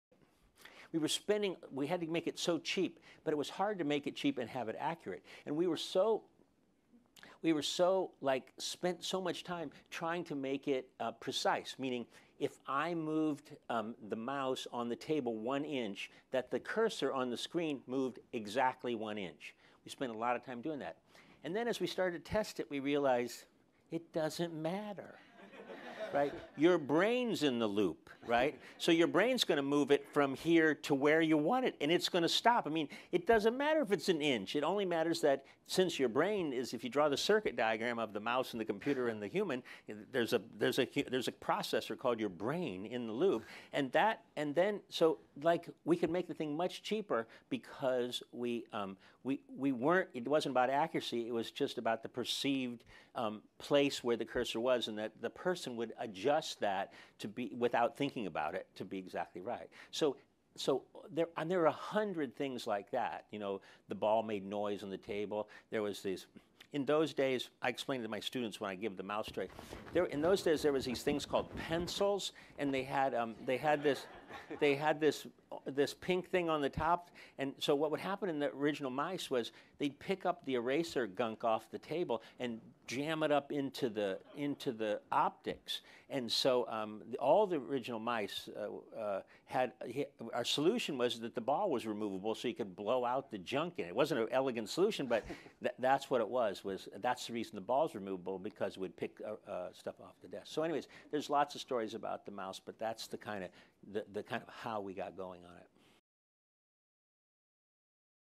David M Kelley on Apple's first mouse
Tom Kelley and David Kelley, who have just published the book "Creative Confidence: Unleashing the Creative Potential Within Us All," have designed a range of extraordinary devices, from Apple's first mouse to a heart defibrillator with voice and visual instructions. As founders of the design firm IDEO, the brothers are now working to improve access to safe drinking water in the developing world. At our Forum, David Kelley talks about his experience designing the Apple mouse.